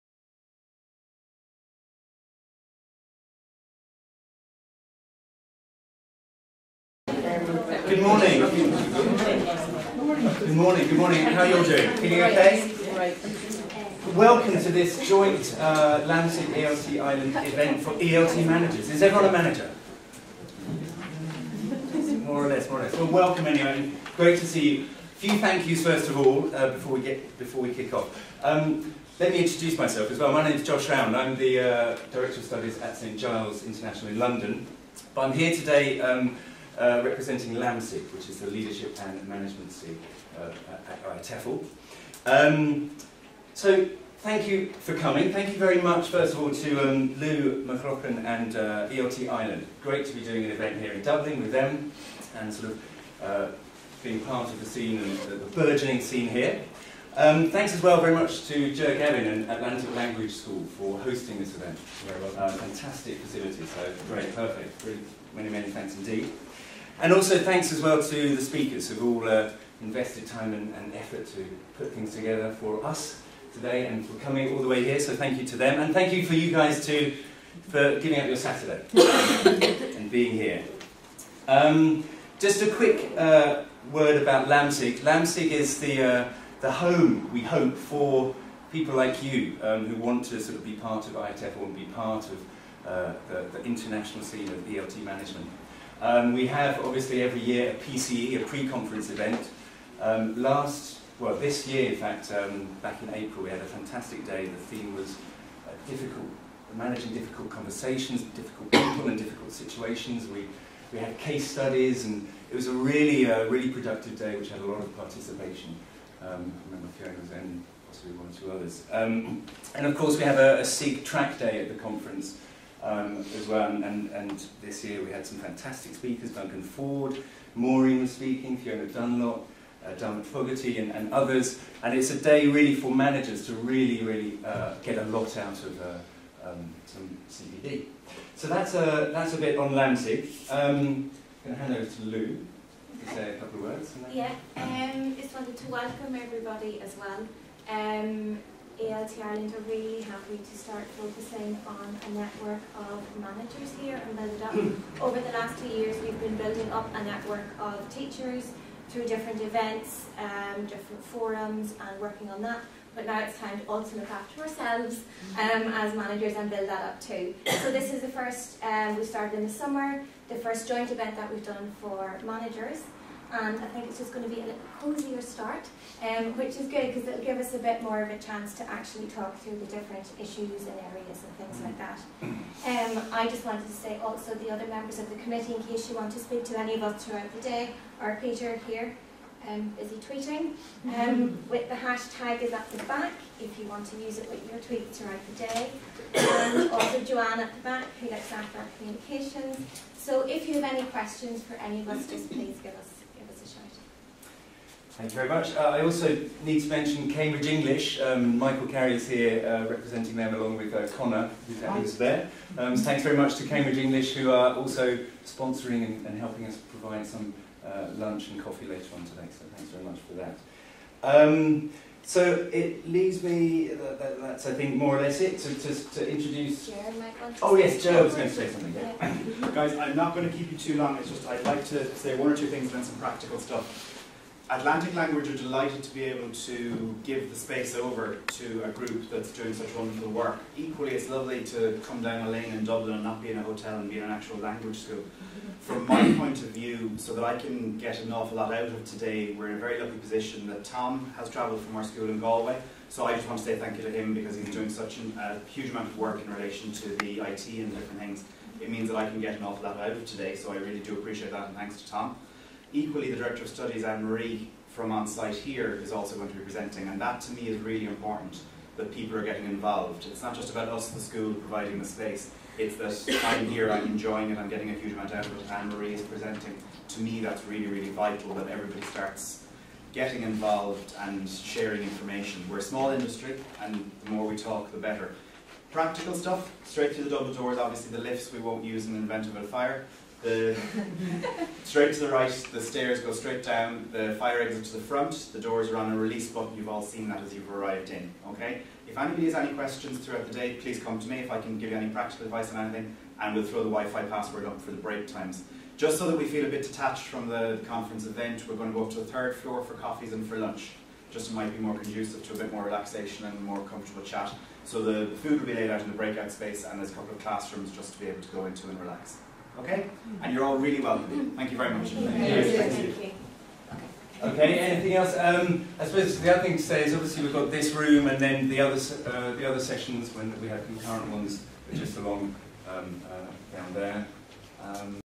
Communication from a Management Perspective a one-day conference from LAMSIG & ELT Ireland
In modern, successful English Language Teaching Organisations, effective communication plays a critical part yet it will always be one of the most difficult and complex skills that the ELT Manager needs to develop. This 1 day event to explore the issues and challenges involved in getting communication right.